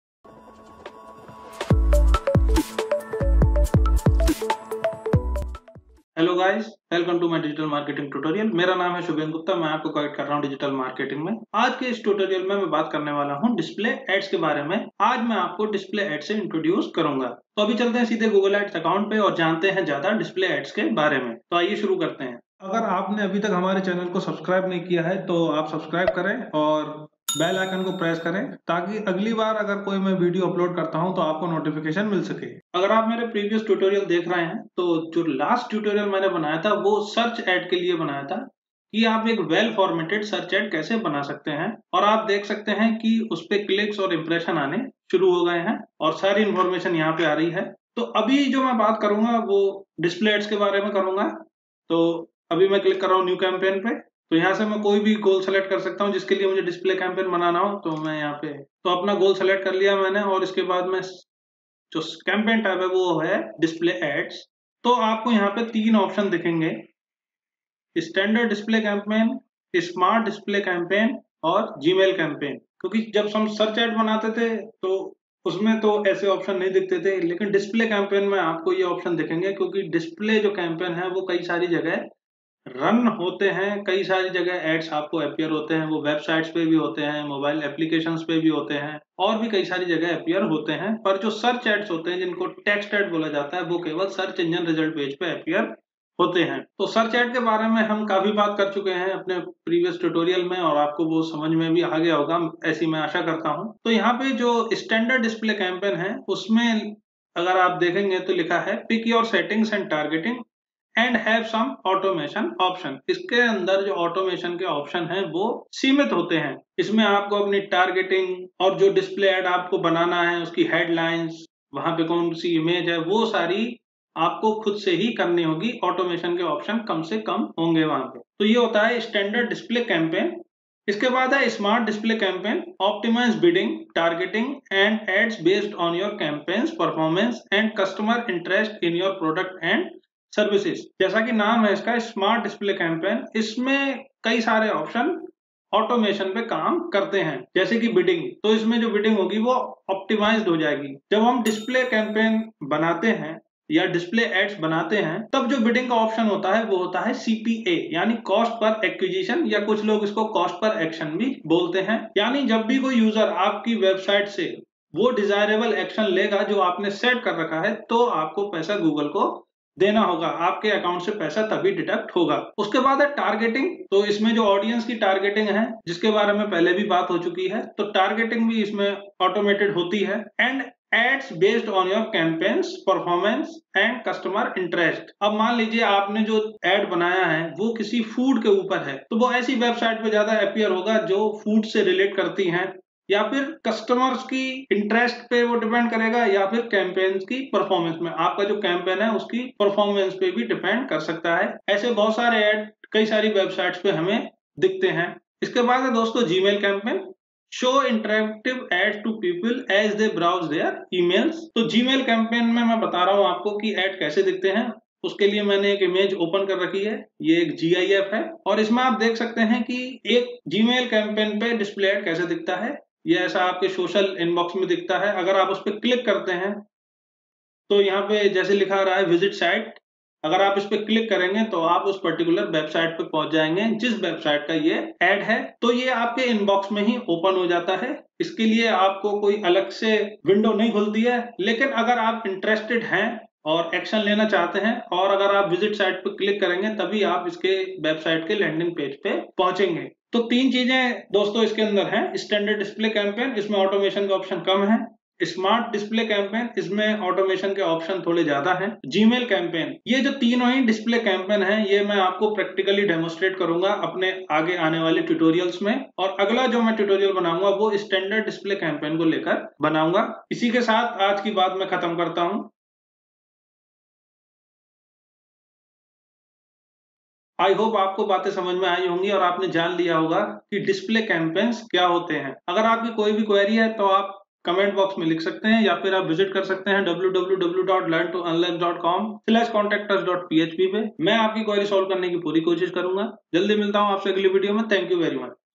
Google Ads Course | Introduction to Display Ads | Hindi
I hope all of you will be good.

Welcome to the twenty-third video of Google Ads Tutorials Series, in this tutorial, I will be going to introduce you Display Ads in Google Ads. If you want to create display ads for various purposes like brand awareness, reach, product consideration etc. then you must watch this tutorial.

Display ads are popular across the globe and many businesses made these ads for brand awareness. Although the CTR is little it can reach 90 % of internet users across the globe.

Google Display Network consists of 20 lakh websites, your ads can appear across a large collection of websites, mobile apps, and video content. Your ads can appear across a large collection of websites, mobile apps, and video content.

After selecting the display campaign type, you have to select the campaign subtype. Three types of subtype available, these are-
1. Standard Display Campaign
2. Smart Display Campaign
3. Gmail Display Campaign

In Standard Display Campaign the automation options are less you have to configure your ad by yourself, targeting, bidding and other settings can be set by you.

In the Smart Display Campaign, the automation options are more, bidding and targeting are automated. The ads are displayed in front of interested customers or based on your campaign’s performance.

In Gmail Display Campaigns ads are shown at the top of your inbox tabs. Some of these ads are expandable. When you click one of these ads, it may expand into an email-sized ad with videos and images. This expandable format can help you meet different advertising goals. When someone clicks an expandable ad, the following clicks on the expanded content are free.

The Google Display Network is designed to help you to find the right audience. Its targeting options let you strategically show your message to interested audience at the right place and the right time.

Watch the tutorial to more about Display ads.

Have a good time ahead!